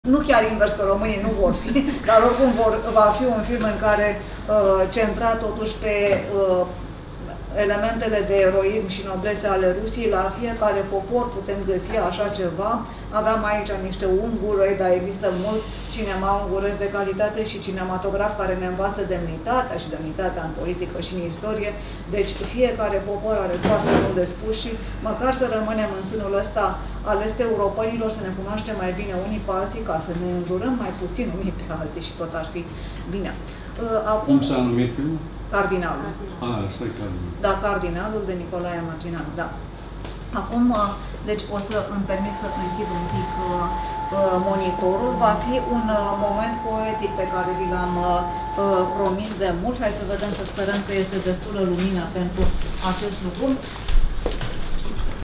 (04) Dr. Elena Dulgheru - Prezentare film Cetatea Novgorodului de Aleksei Saltîkov (2022.08.20)
În 20 august 2022 a avut loc o prezentare selecție de filme istorice "Eroismul creștin și patriotic - fundament al identității naționale" și alte momente culturale la Curtea Brâncovenească.

Dr. Elena Dulgheru a făcut o prezentare a filmelor:
 - "Cardinalul", în regia lui Nicolae Mărgineanu;
 - "Cetatea Novgorodului", în regia lui Aleksei Saltîkov.

La eveniment a avut loc și un recital poetic ("România voievodală"), cu lecturi din "Erotico-Apocaliptica" (dr. Elena Dulgheru). A fost citită și o poezie a d-lui Ion Hîrbu.